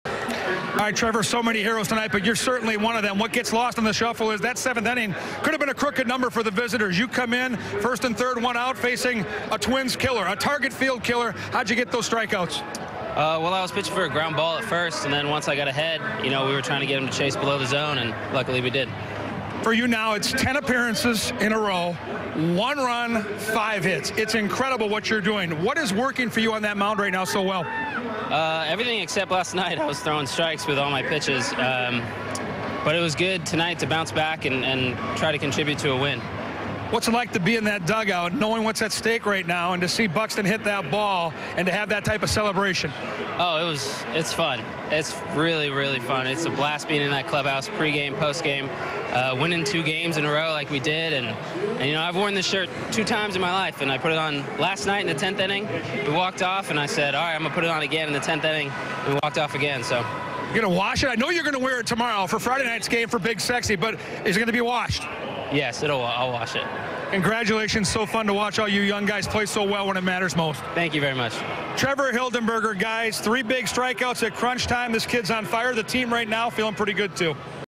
Hildenberger's lucky shirt doing big things for Twins
Trevor Hildenberger says he's worn this shirt twice. Both times, the Minnesota Twins have won on a walk-off home run in extra innings.